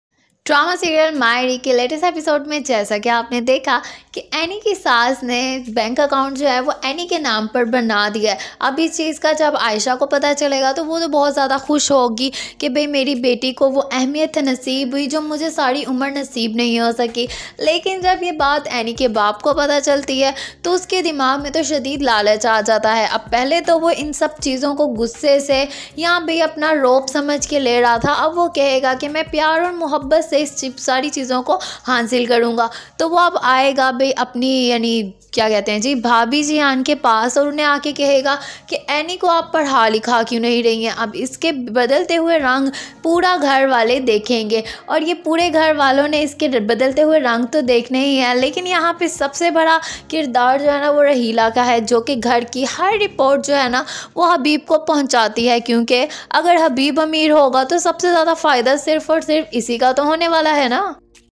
New! Mayi Ri episode 51 & 52 promo review - Fakhar and Annie loved each other 💘 😇 - Sept 21, 2023
New! Mayi Ri Episode 51 & 52 | Promo review by Maria Javed 1M views | Aina Asif | Samar Abbas | Naumaan Ijaz | Maya Khan | Maria Wasti |

21st September ,2023 | ARY Digital Drama

Cast:

Aina Asif

Samar Abbas

Naumaan Ijaz

Maria Wasti

Maya Khan

Sajida Syed

Saad Faridi

Amna Malik

Paras Masroor Usman Mazhar

Diya Mughal

Directed By: Meesam Naqvi Concept & Story: Sana Fahad